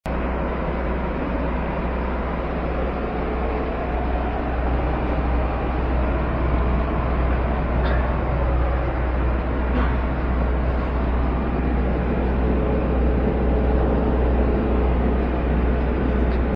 Tarmack Tram To Customs PS 9262 to KBP Kiev Ukraine
Arrive KBP Tarmack Tram To Customs
PS9262 CDG Paris, France to KBP Kyiv, Ukraine
*Operating as Air France 1752
Ukraine International Airlines
BOEING 737-900ER with winglets